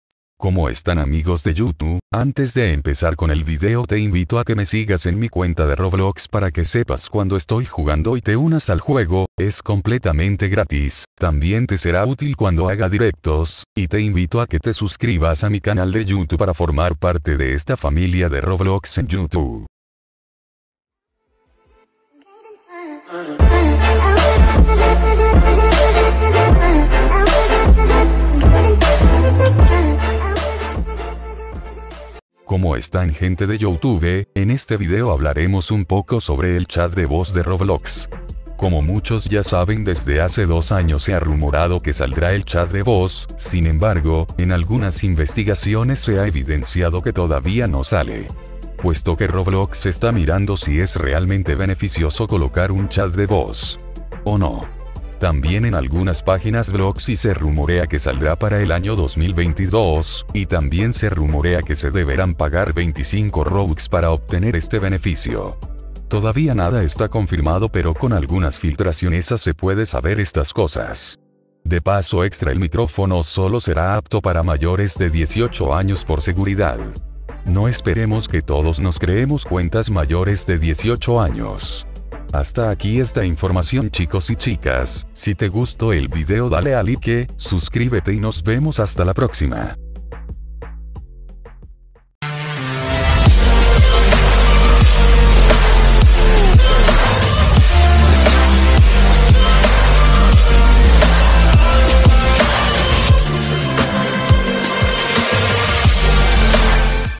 Cuando Sale El CHAT De VOZ DE ROBLOX??(novedades)
Como están chicos y chicas, en este video se presenta el nuevo chat de voz que será para todos los usuarios de Roblox.

INFORMACION AQUI

❤️ ★▬Primer@ En Comentar En El Video Anterior▬★❤️